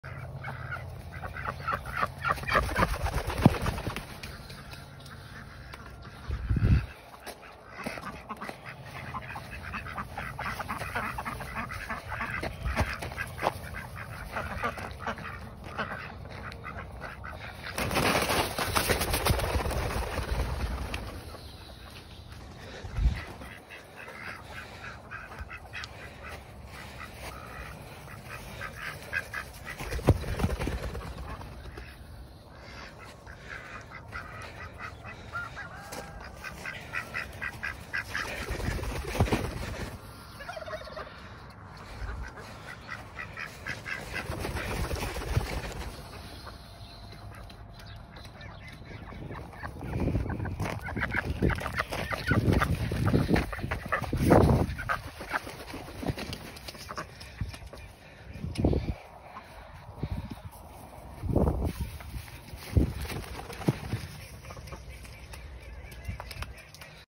Ducks flapping and skating on ice
Domestic duck “flying” exercises